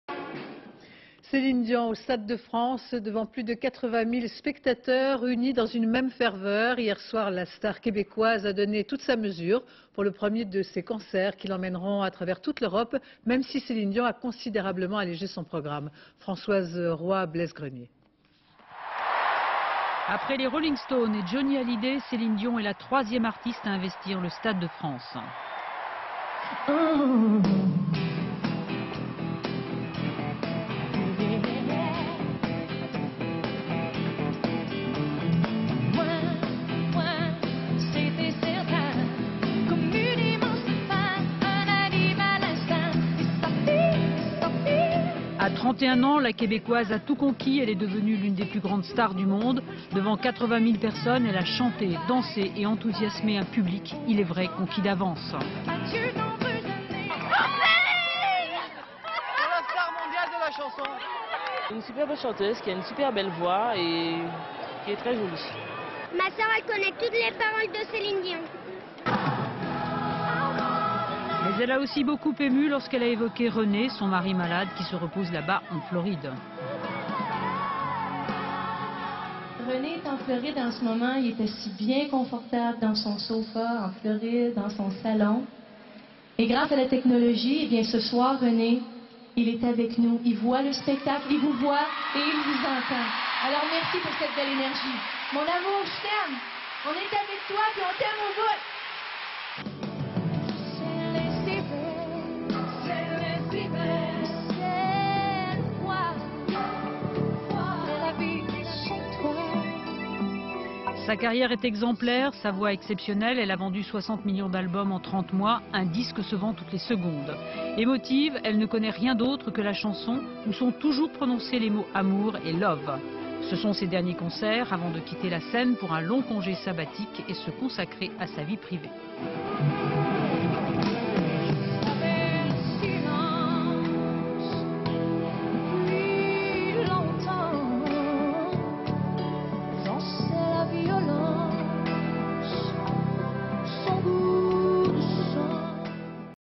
Céline Dion en concert au Stade de France | Archive INA
20 juin 1999
Reportage illustrant le concert donné par Céline DION au Stade de France devant plus de 80000 spectateurs. Images d'archive INA